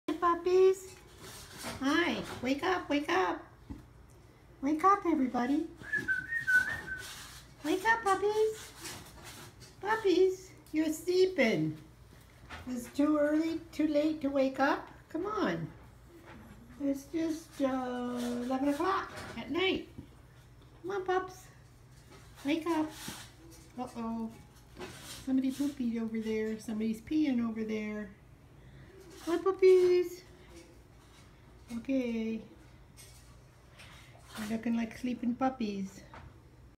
Icepond Carly Waylon pups 8 weeks
Waking up to go out one last time before nite nite